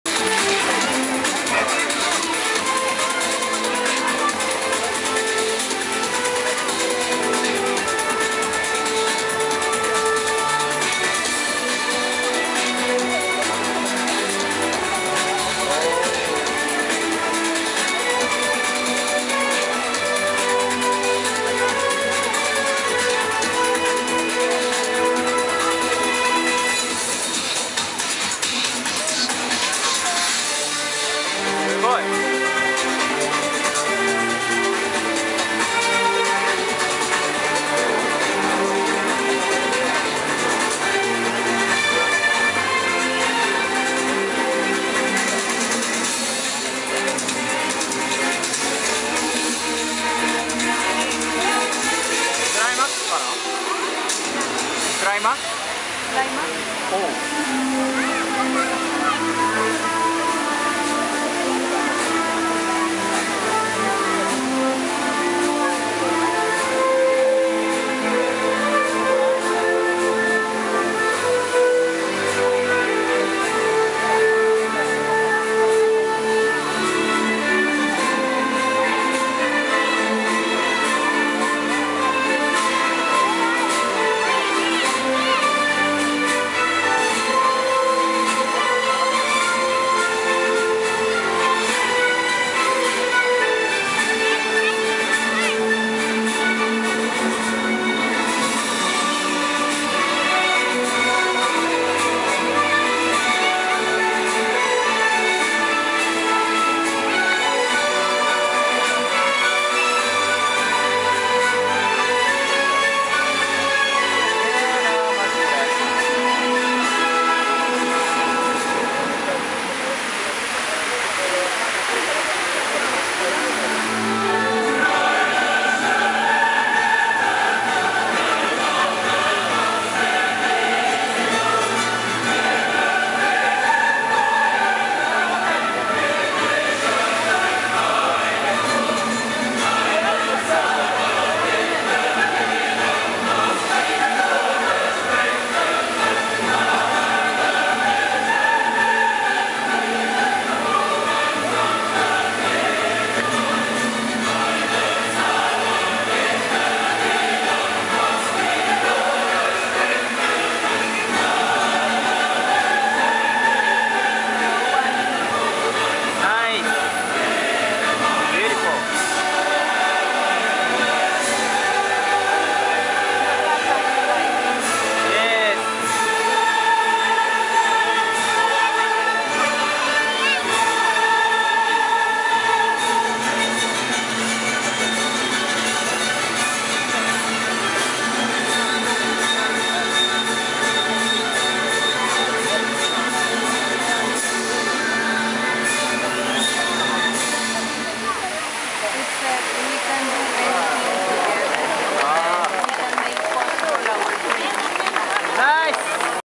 Amazing Water fountain SHOW!! in PERU!!
You should go to LIMA!!(Capital of PERU)

from HIDEKI OTA
I'm traveling around the world.